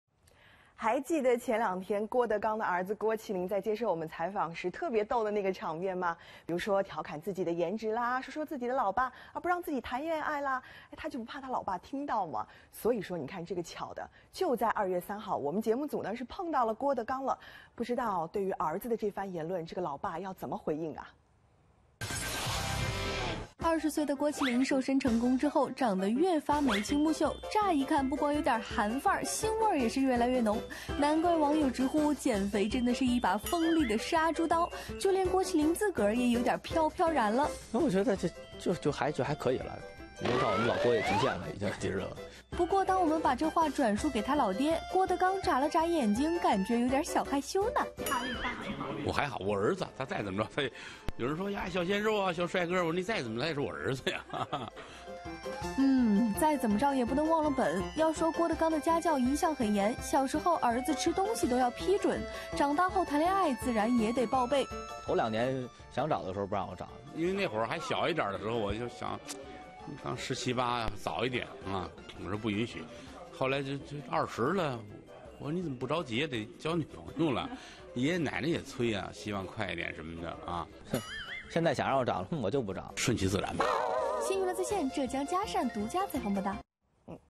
看看星闻 | 郭德纲：儿子再帅也是我儿子
【新闻摘要】近日，郭德纲晒出自己年满20岁的大儿子郭麒麟，照片中的郭麒麟成功瘦身手拿玫瑰帅气十足，获赞颜值逆天新晋男神。